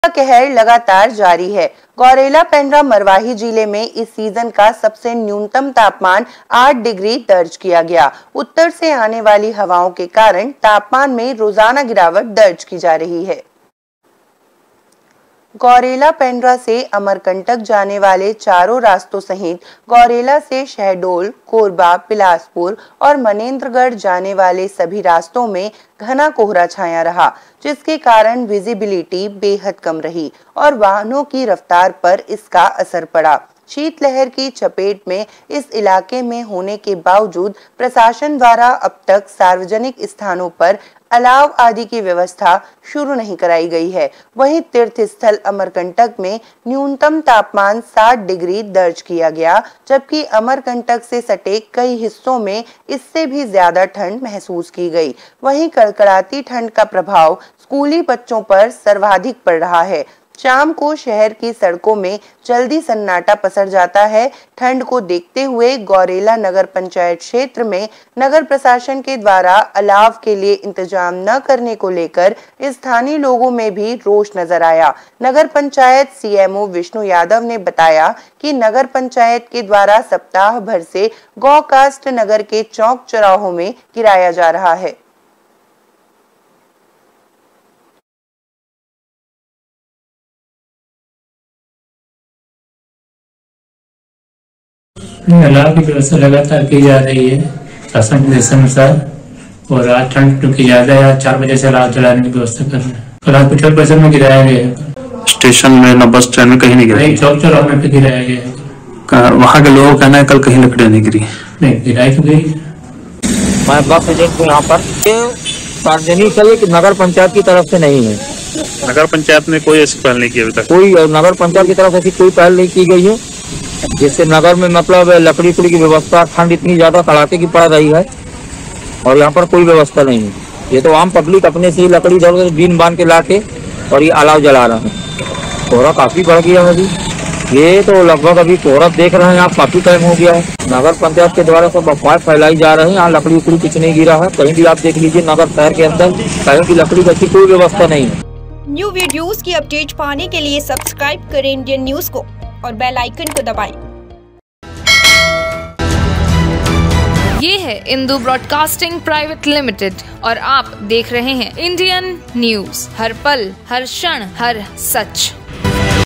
गौरेला-पेंड्रा-मरवाही जिले में कड़ाके की ठंड : न्यूनतम तापमान 8 डिग्री सेल्सियस की दर्ज
Indian news : ठंड का कहर लगातार  जारी है, गौरेला- पेंड्रा- मरवाही जिले में इस सीजन का सबसे न्यूनतम तापमान 8 डिग्री दर्ज किया गया।उत्तर से आने वाली हवाओं के कारण तापमान में रोजाना गिरावट दर्ज की जा रही है
गौरेला पेंड्रा से अमरकंटक जाने वाले चारों रास्तों सहित गौरेला से शहडोल, कोरबा, बिलासपुर और मनेन्द्रगढ़ जाने वाले सभी रास्तों में घना कोहरा छाया रहा । जिसके कारण विजिबिलिटी बेहद कम रही और वाहनों की रफतार पर इसका असर पड़ा। शीतलहर की चपेट में इस इलाके में होने के बावजूद प्रशासन द्वारा अब तक सार्वजनिक स्थानों पर अलाव आदि की व्यवस्था शुरू नहीं करायी गयी है। वहीं तीर्थ स्थल अमरकंटक में न्यूनतम तापमान 7 डिग्री दर्ज किया गया जबकि अमरकंटक से सटे कई हिस्सों में इससे भी ज्यादा ठंड महसूस की गयी। वहीं कड़कड़ाती ठंड का प्रभाव स्कूली बच्चों पर सर्वाधिक पड़ रहा है। शाम को शहर की सड़कों में जल्दी सन्नाटा पसर जाता है।ठंड को देखते हुए गौरेला नगर पंचायत क्षेत्र में नगर प्रशासन के द्वारा अलाव के लिए इंतजाम न करने को लेकर स्थानीय लोगो में भी रोष नजर आया। नगर पंचायत सीएमओ विष्णु यादव ने बताया कि नगर पंचायत के द्वारा सप्ताह भर से गौ कास्ट नगर के चौक चौराहों में गिराया जा रहा  है ।

Our Links